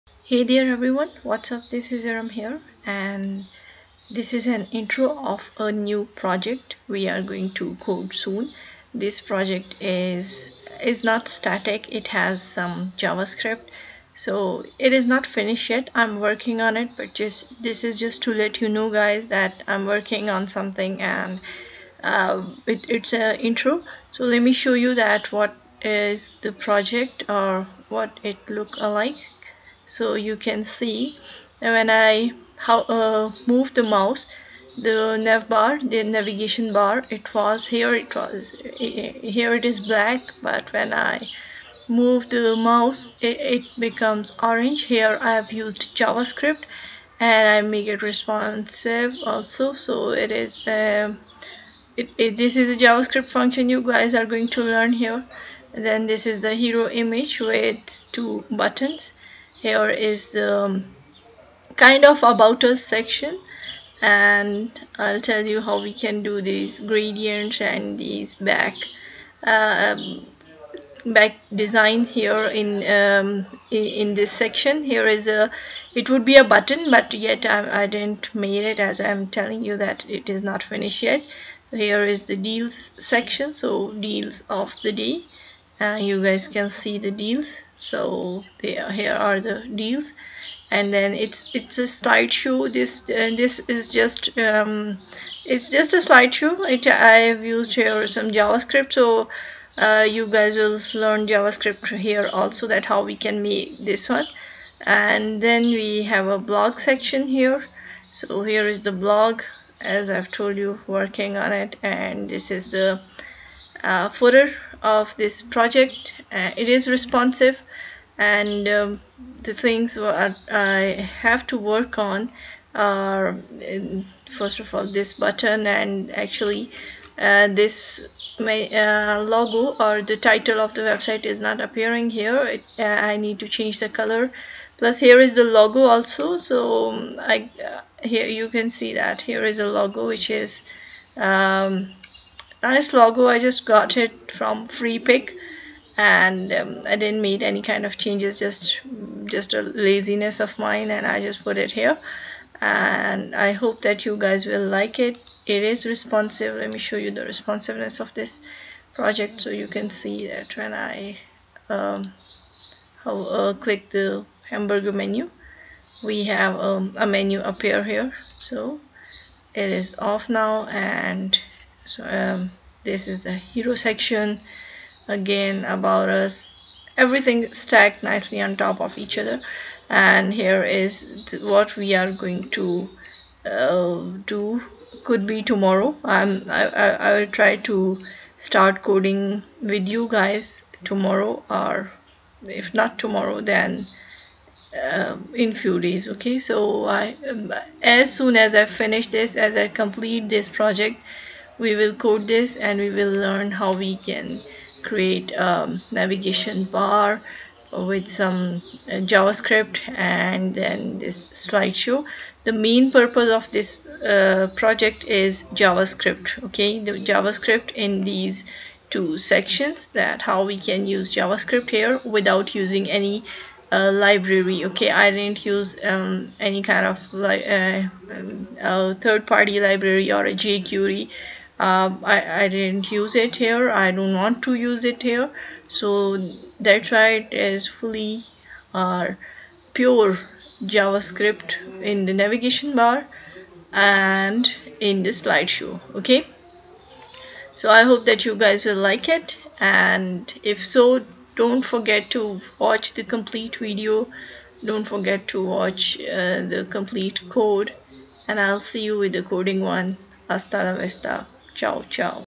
JavaScript Project Intro | Restaurant Website Development using JavaScript
This is the introduction of a javascript project. In this project, we are going to use a javascript navigation bar and image slider which will be fully responsive. We are not going to use any Jquery it is pure javaScript in these sections.